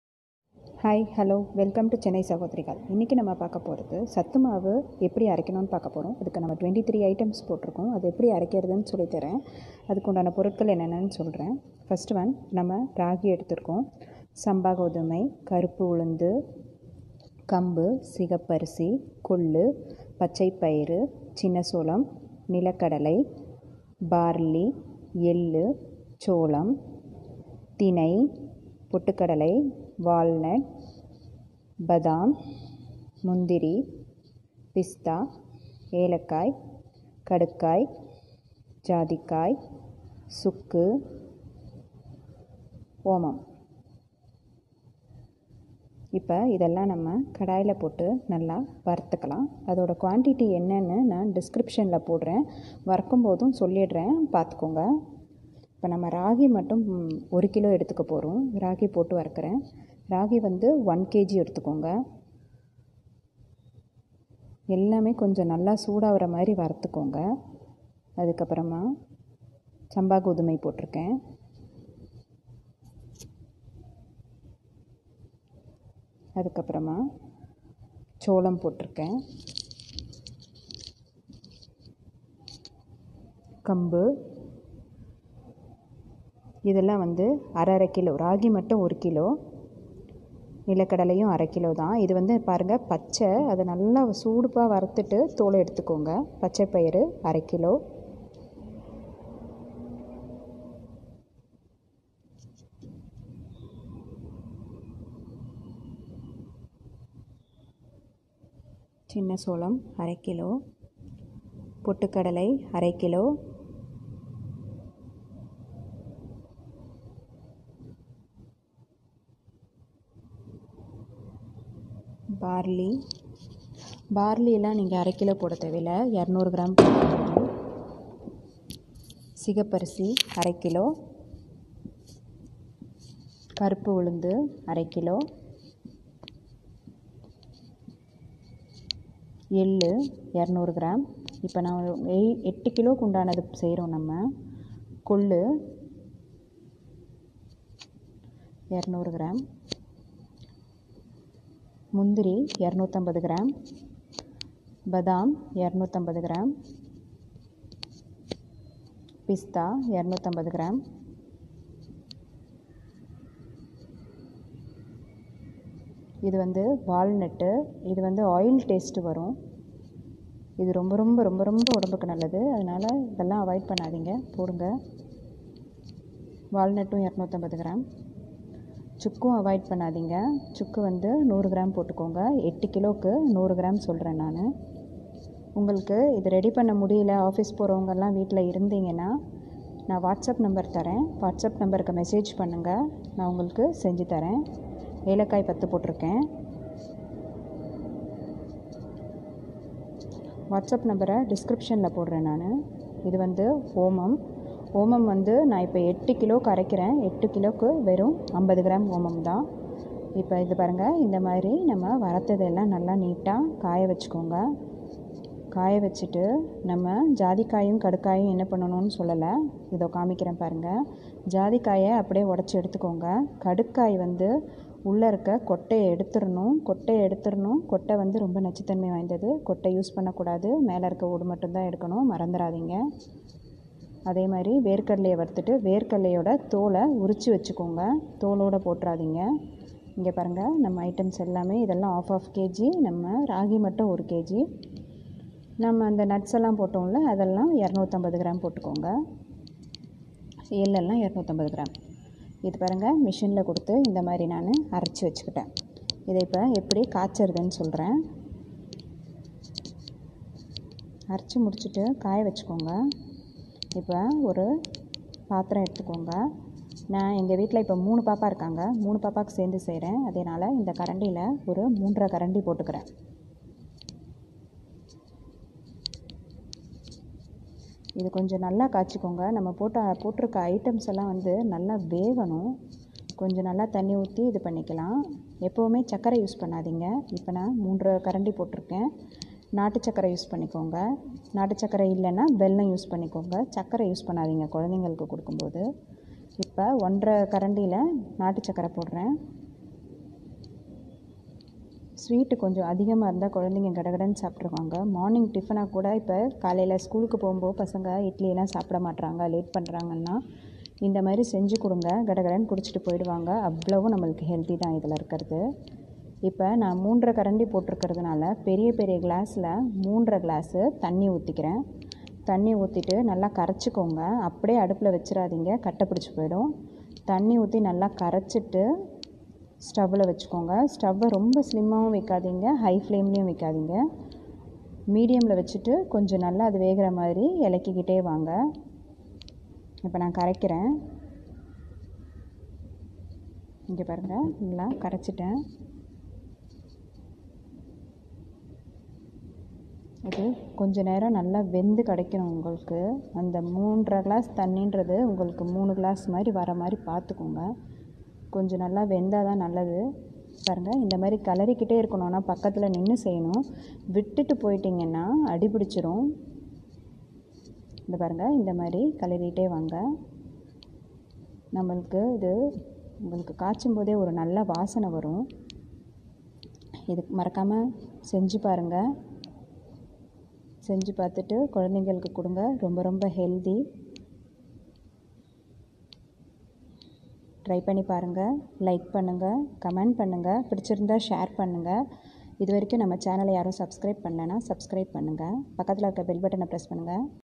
சத்து மாவு வீட்டில் அரைக்கும் முறை / குழந்தைகள் முதல் வயதானவர்கள் வரை குடிக்கலாம் இந்த சத்து மாவு
சத்து மாவு வீட்டில் அரைக்கும் முறை / குழந்தைகள்  முதல் வயதானவர்கள் வரை குடிக்கலாம்  இந்த சத்து மாவு / how to prepare health mix at home / சத்து மாவு செய்வது எப்படி தமிழில்.

ஒரு டம்பளர் குடித்தால் ஒரு நாள் முழுவதும் சுறுசுறுப்போடு இருப்பீர்கள் . அன்றைய தினத்திற்கு தேவையான ஊட்டச்சத்து முழுவதும் கிடைத்துவிடும்.

 This health mix gives energy for the whole day.

சத்து  மாவு அரைப்பதற்கு தேவையான பொருட்கள்

ராகி                                       -            1 KG,
சம்பா கோதுமை          -      1/2 KG,
கருப்பு உளுந்து                -            1/2 KG,
கம்பு                                      -      1/2 KG,
சிகப்பரிசி                   -      1/2 KG,
 நிலக்கடலை                  -      1/2 KG,
 சின்ன சோளம்          -      1/2 KG,
பச்சை பயறு              -      1/2 KG,
சோளம்                  -      1/2 KG,
திணை                          -      1/2 KG,
 பொட்டுக்கடலை           -      1/2 KG,
எள்ளு                          -      200 Grams,
கொள்ளு                   -       200 Grams,
பார்லி                           -       200 Grams
பிஸ்தா                   -             250 Grams
முந்திரி                   -       250 Grams
பாதாம்                   -        250 Grams
வால்நட்                   -        250 Grams
ஏலக்காய்                   -        10 nos.
ஜாதிக்காய்           -         2 nos.
கடுக்காய்                   -         2 nos.
சுக்கு                    -        100 Grams
ஓமம்                   -            50 Grams

செய்முறை:

எல்லா பொருட்களையும் வறுத்து ஆறவைத்து பின்பு மாவுக்கடையில் கொடுத்து அரைத்து கொள்ள வேண்டும்.
பின்பு அதை காய்ச்சி குடிக்கலாம் முதியவர்களும் குடிக்கலாம்.

தெளிவாக வீடியோ வில் கொடுத்துள்ளேன் பாருங்கள்.